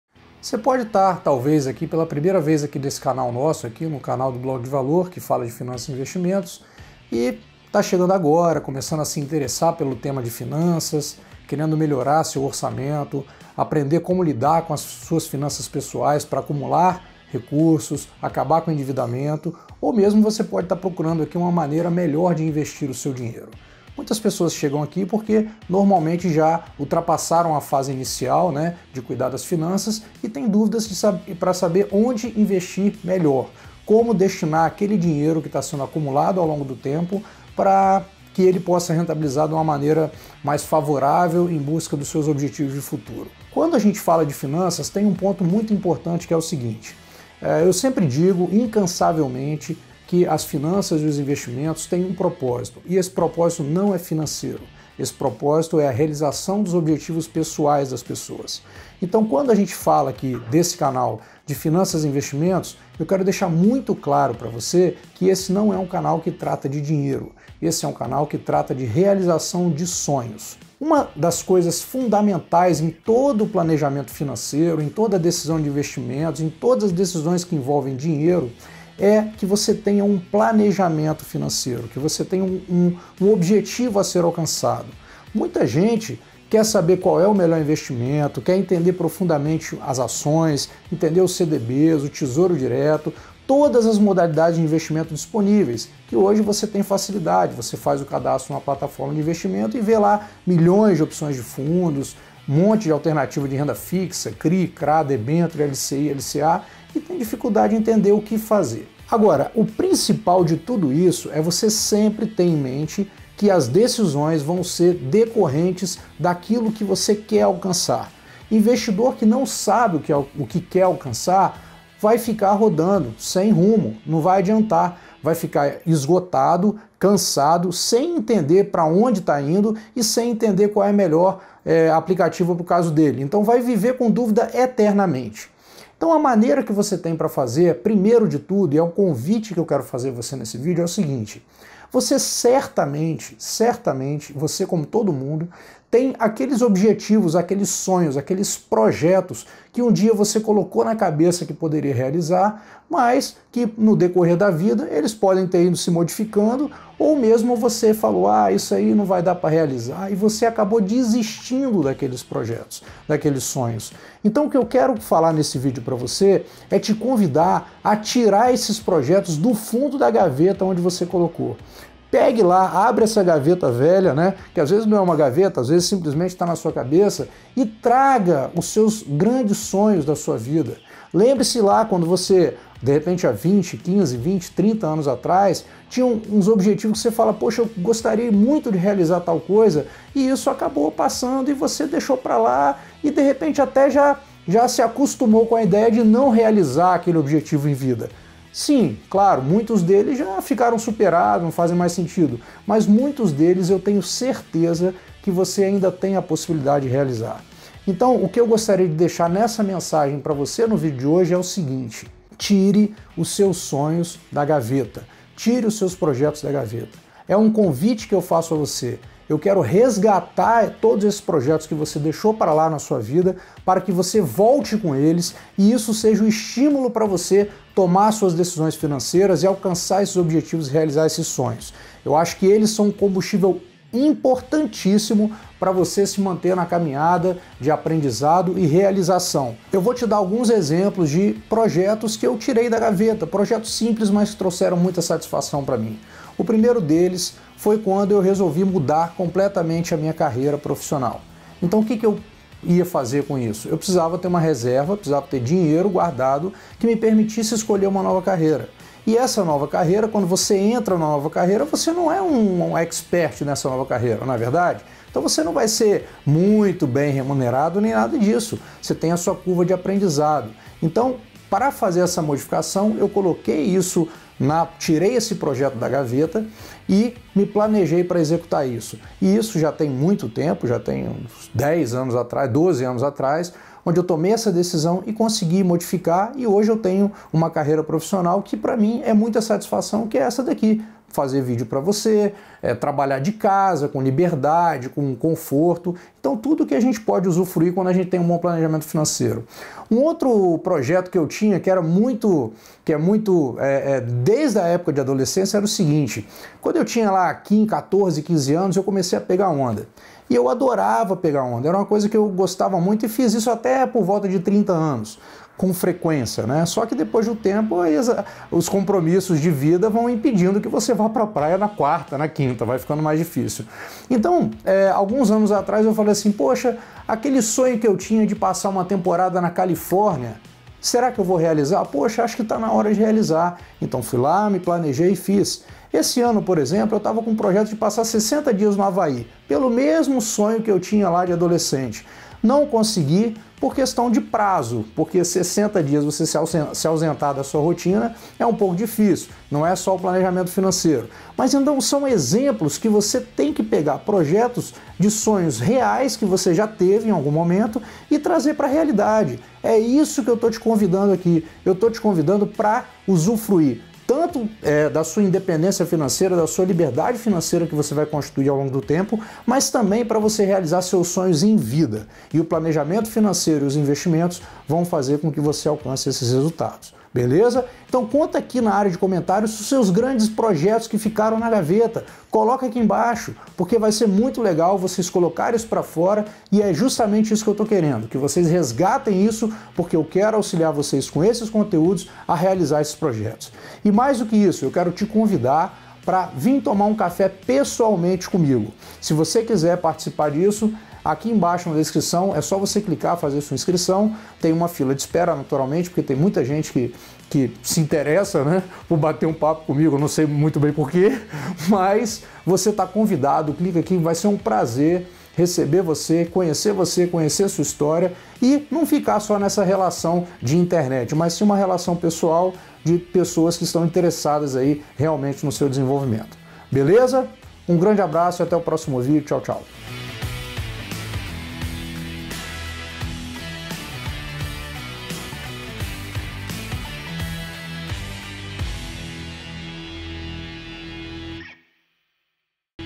Tire seus sonhos da gaveta! Use a educação financeira e o mercado financeiro com sabedoria!
► Quer tomar um café comigo? Veja participar do Café com Bona!

Se engana quem pensa que educação financeira e investimentos trata de dinheiro. Não trata disso.

Educação financeira trata das possibilidades de uma pessoa viabilizar sua felicidade. Trata da capacidade e viabilidade de realizar sonhos, de ser feliz! A prosperidade é um caminho que pode auxiliar nesse processo.

O convite que te faço nesse vídeo é o de tirar seus sonhos de gaveta. Pegue aqueles sonhos que ficaram para trás, mas que você ainda gostaria de realizar. Tire-os de lá, de onde estão escondidos. Traga os para fora. Coloque-os novamente nas suas metas. Eles podem ser o combustível e a motivação para que você possa utilizar o mercado financeiro com sabedoria.

Ganhar dinheiro, juntar dinheiro, tudo isso é importante. E essa jornada nada mais é do que um processo de autoconhecimento. Entender suas prioridades em busca do seu bem estar e, até mesmo, a sua espiritualidade, a sua sabedoria!

Não estamos falando de dinheiro. Estamos falando de viver a vida e realizar nossos projetos! Aqueles mais audaciosos!